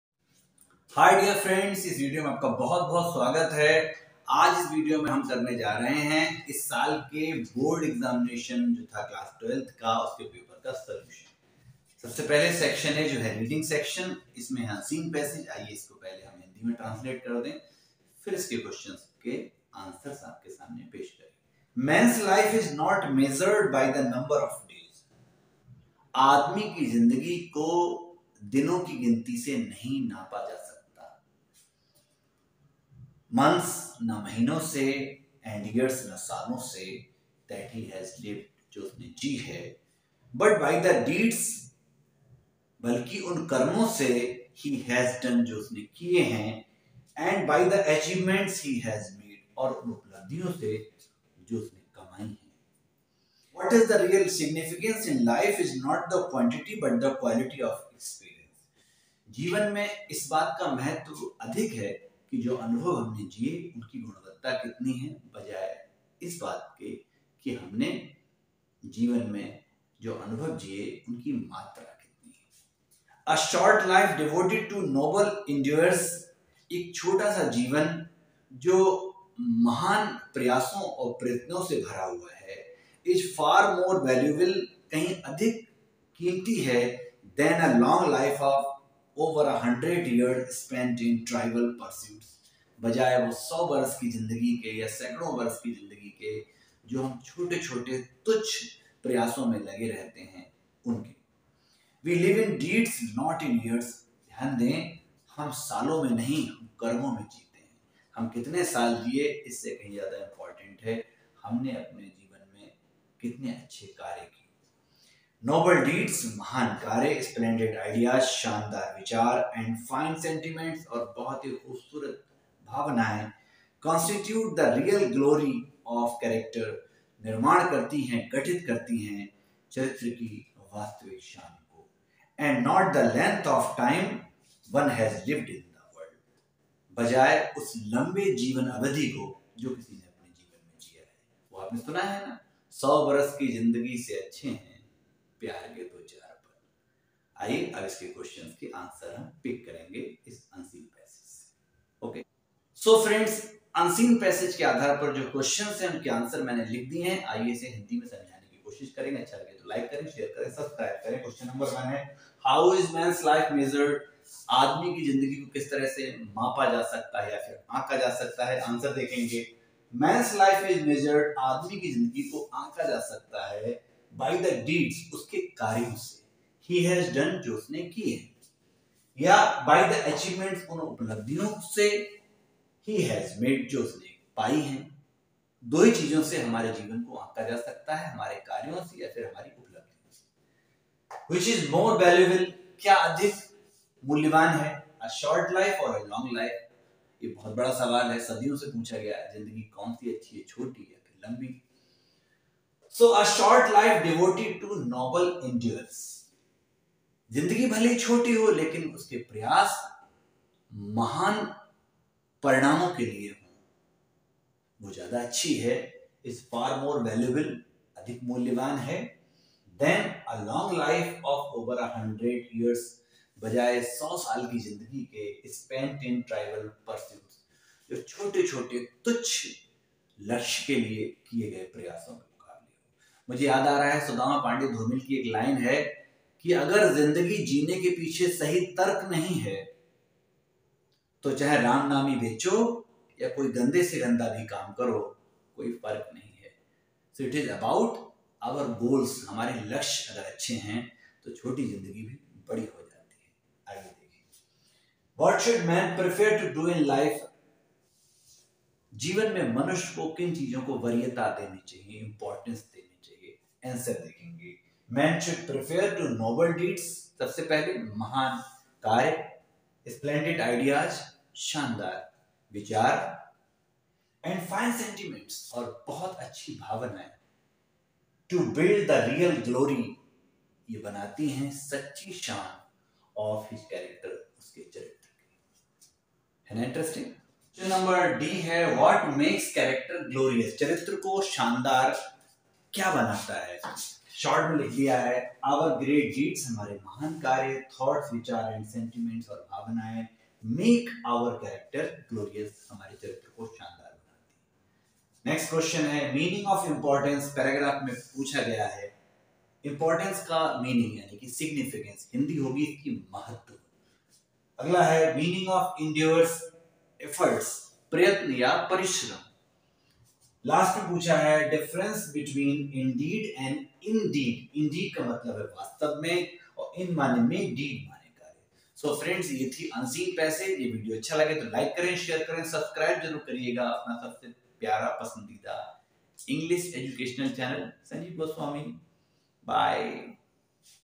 Unseen Passage/Board Examination Upboard Solution 2022/Class 12
Solution Paper of Upboard Examination 2022, Class 12
Dear friends,
you will get the solution of unseen passage of Upboard Examination paper class 12 this year.